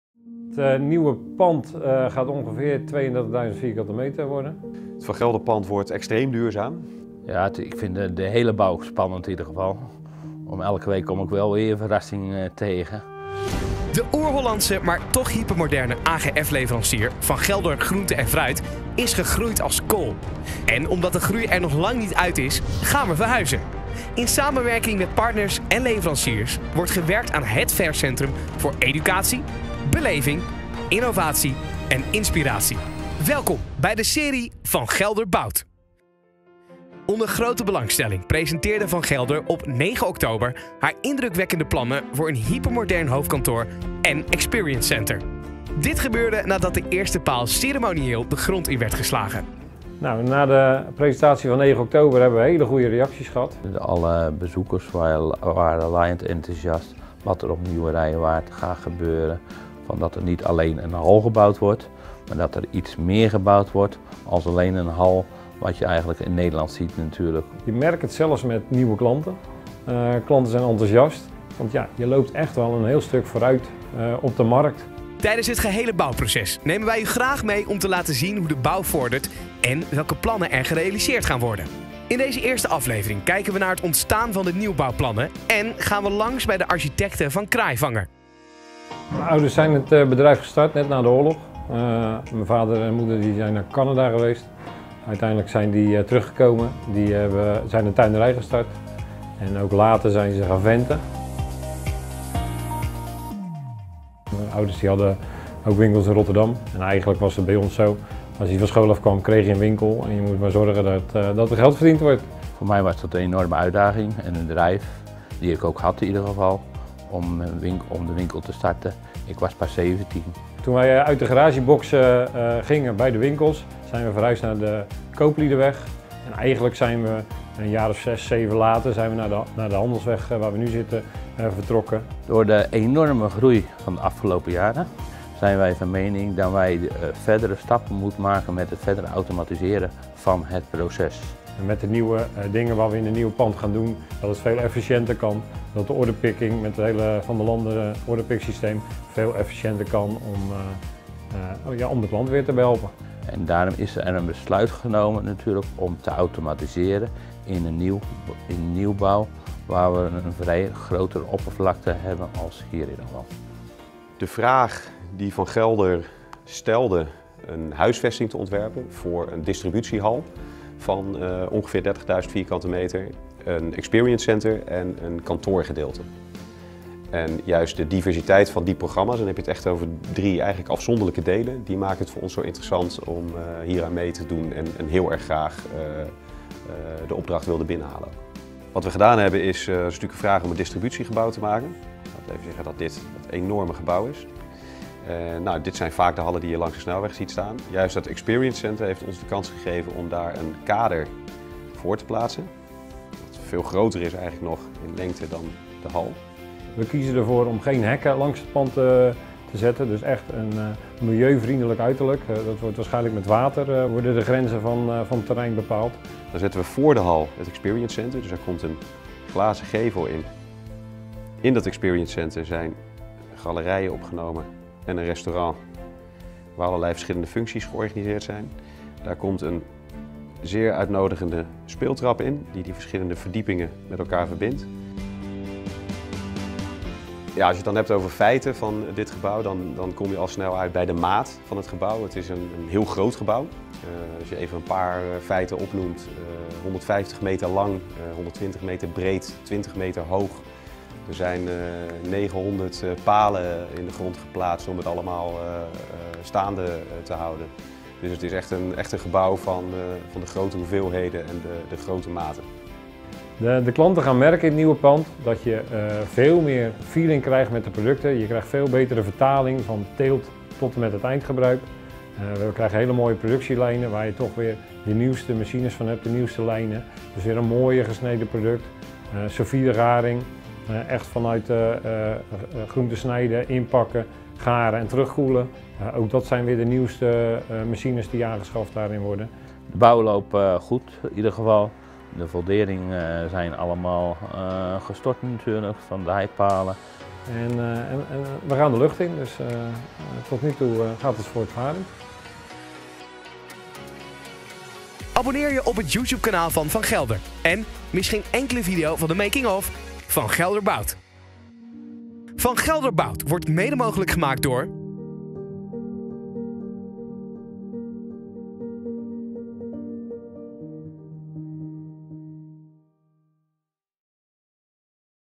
Van Gelder bouwt #01 We gaan de hoogte in!
Onder grote belangstelling presenteerde Van Gelder op 9 oktober haar indrukwekkende plannen voor een hypermodern hoofdkantoor en Experience Center. Sindsdien hebben we niet stil gezeten en is er op de bouwplaats hard gewerkt.